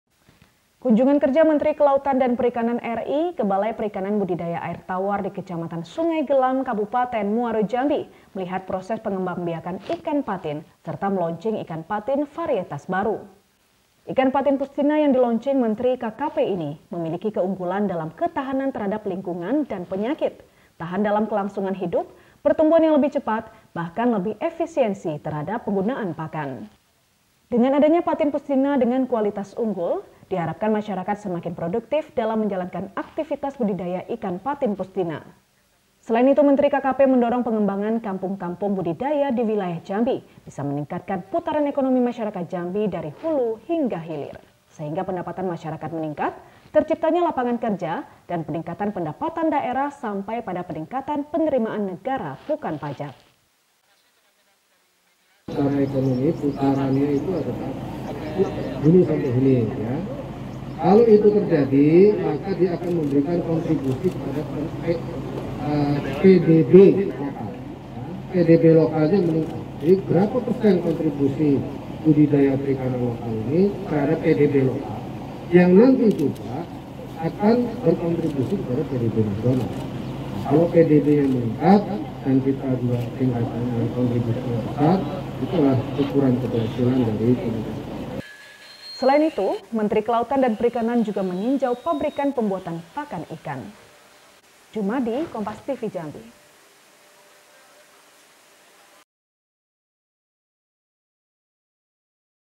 Menteri KKP Launching Ikan Patin Pustina
MUARO JAMBI, JAMBI, KOMPAS.TV - Kunjungan kerja Menteri Kelautan dan Perikanan RI ke balai perikanan budidaya air tawar di Kecamatan Sungai Gelam Kabupaten Muaro Jambi melihat proses pengembang biakan ikan patin serta melaunching ikan patin varietas baru. Ikan patin pustina yang di launcing Menteri KKP ini memiliki keunggulan dalam ketahanan terhadap lingkungan dan penyakit, tahan dalam kelangsungan hidup pertumbuhan yang lebih cepat bahkan lebih efisiensi terhadap penggunaan pakan.

Dengan adanya patin pustina dengan kualitas unggul diharapkan masyarakat semakin produktif dalam menjalankan aktivitas budidaya ikan patin pustina. Selain itu Menteri KKP mendorong pengembangan kampung-kampung budidaya di wilayah Jambi bisa meningkatkan putaran ekonomi masyarakat Jambi dari hulu hingga hilir. Sehingga pendapatan masyarakat meningkat terciptanya lapangan kerja dan peningkatan pendapatan daerah sampai pada peningkatan penerimaan negara bukan pajak.